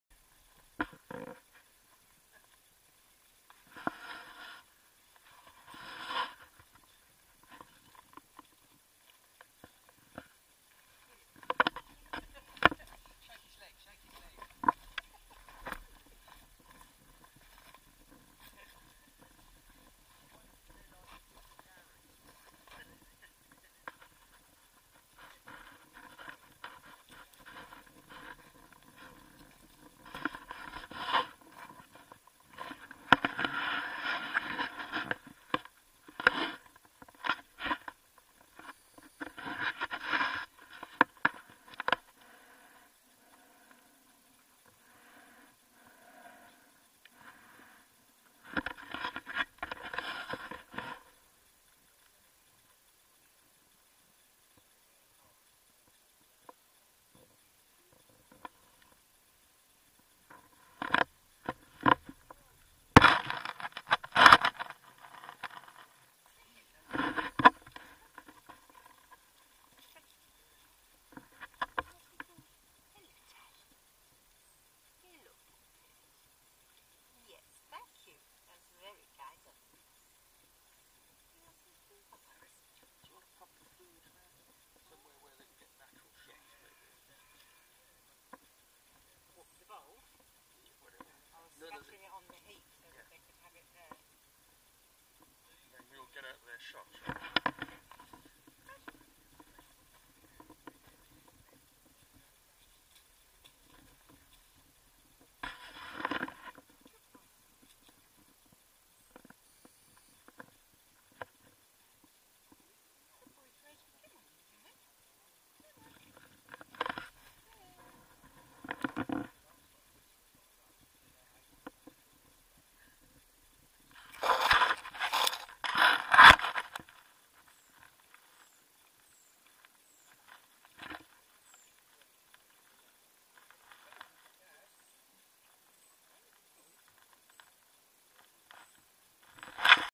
Badgering around at Wildwood Trust
Its time to badger around with our badger cam - get a badger eyes view of the world...